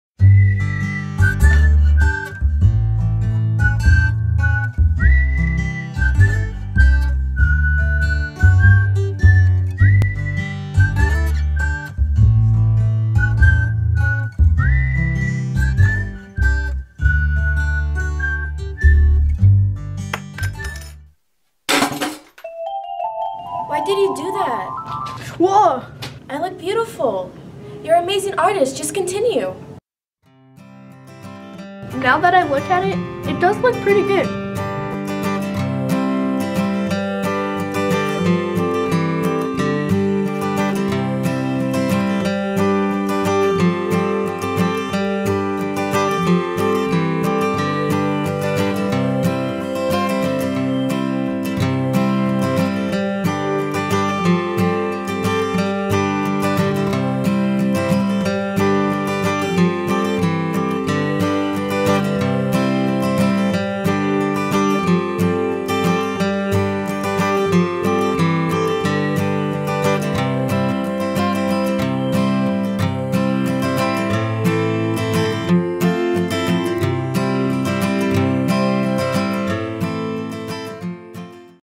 "The Inspired Artist" -- Activating Change
A struggling artist gets motivating to keep working on his dream of creating a masterpiece. Created by Lauffer Middle School Students in Tucson, Arizona for the Global Student Voice Film Festival for the theme of "activating change."